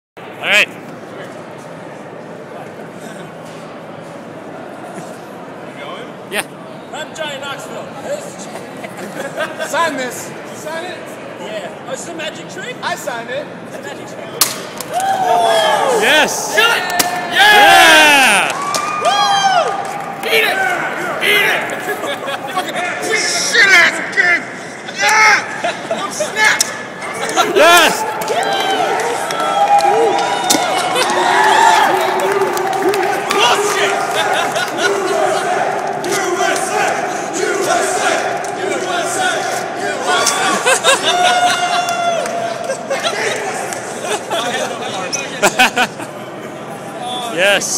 Egoraptor destroys Sonic '06 - MAGfest 2013
What could have made my already awesome MAGfest even more awesome? How about having Egoraptor destroy my copy of Sonic '06!